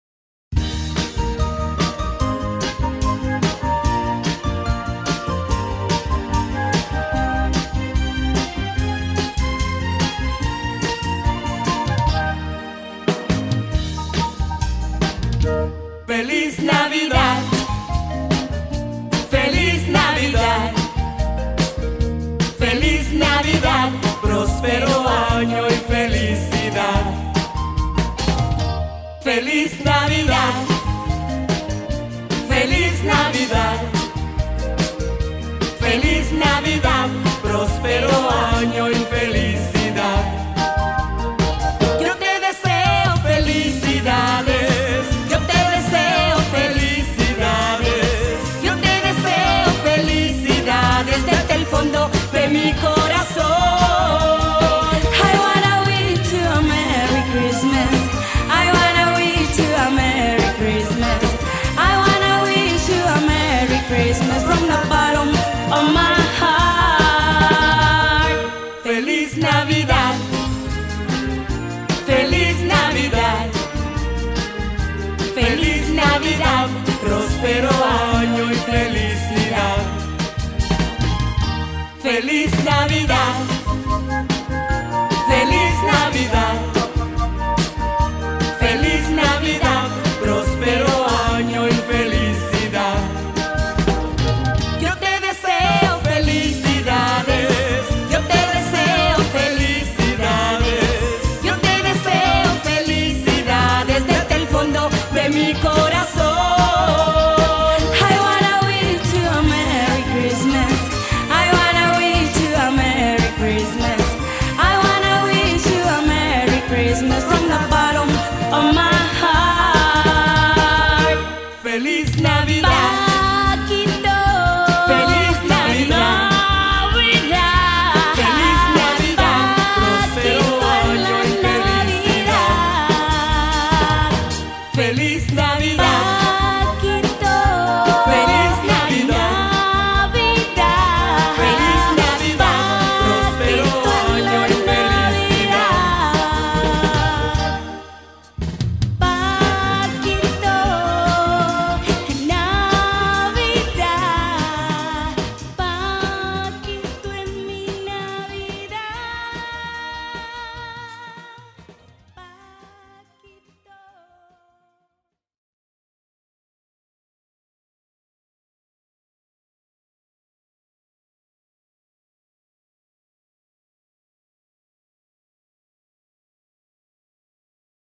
Paquito Chapoy Bazar Navideño 2010.mpg
video paquito chapor relizado especialmente para el opening del Bazar Navideño que organiza DIF Sonora en su edición 2010